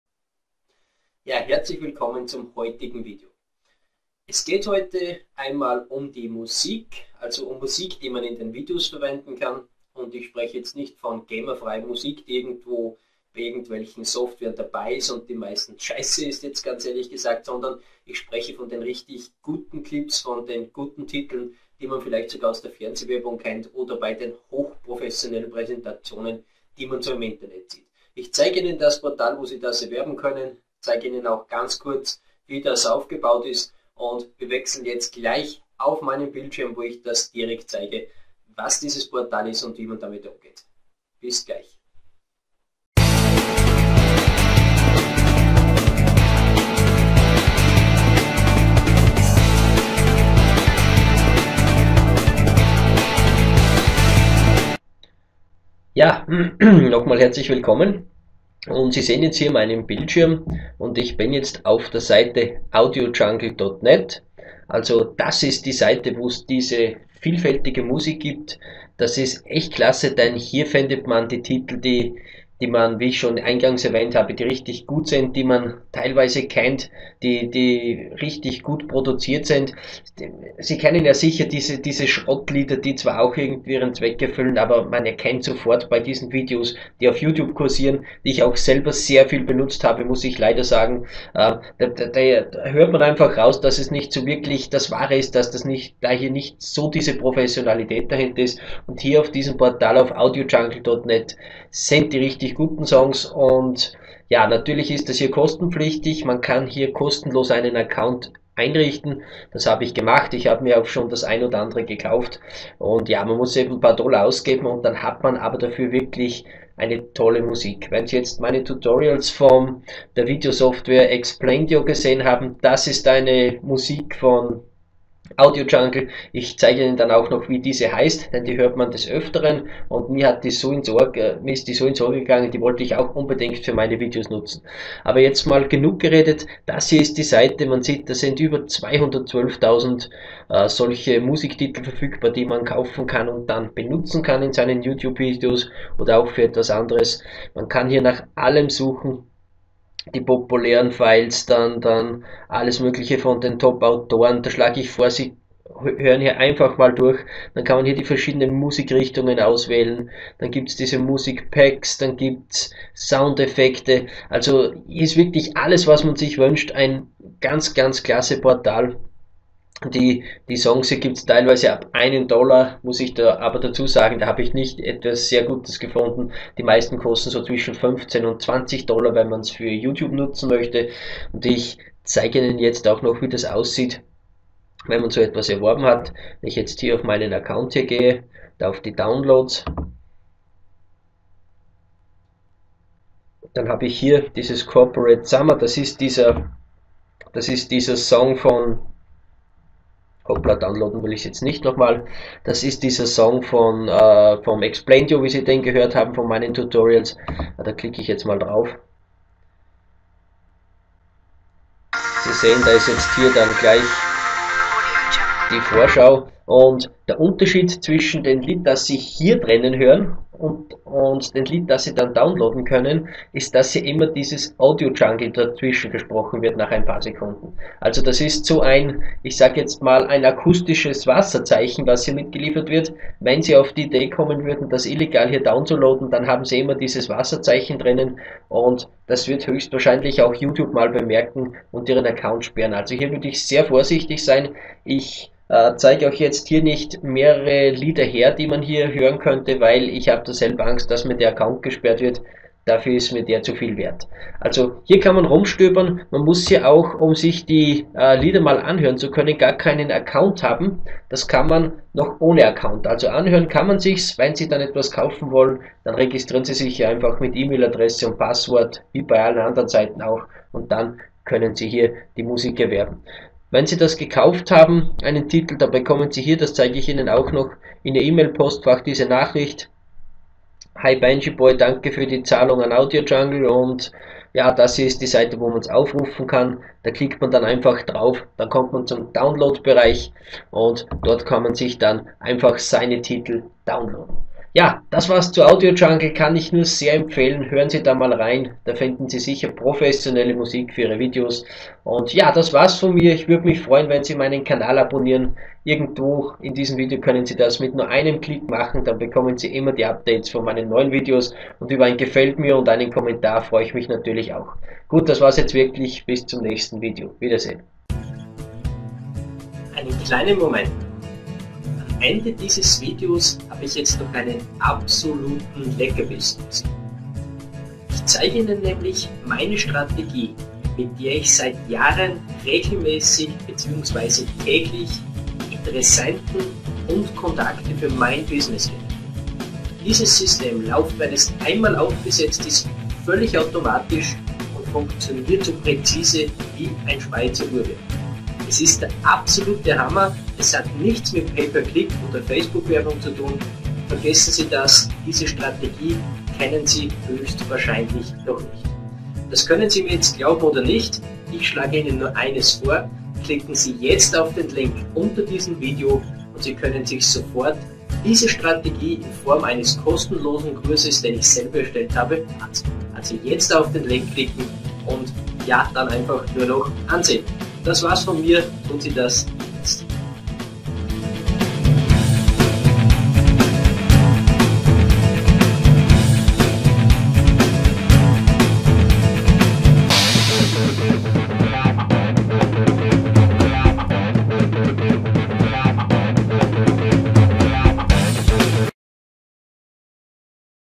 Gemafreie Musik downloaden bei Audiojungle
Gemafreie Musik downloaden bei Audiojungle. Oft werden ja kostenlose Titel mit gewissen Software und anderen Tools mitgeliefert.

Zum Beispiel bei Camtasia Studio ist Musik dabei auch beim Video Maker FX und Explaindio sind solche Titel dabei. Meistens sind aber solche Gemafreien Songs die irgendwo kostenlos zum Downloaden sind nicht wirklich gut. Man kennt einfach den Unterschied.

Bei Audiojungle gibt es die wirklich guten Songs. Da muß man zwar bezahlen aber dann hat man eben tolle Gemafreie Musik die man mit ziemlicher Sicherheit teilweise aus der Fernsehwerbung kennt.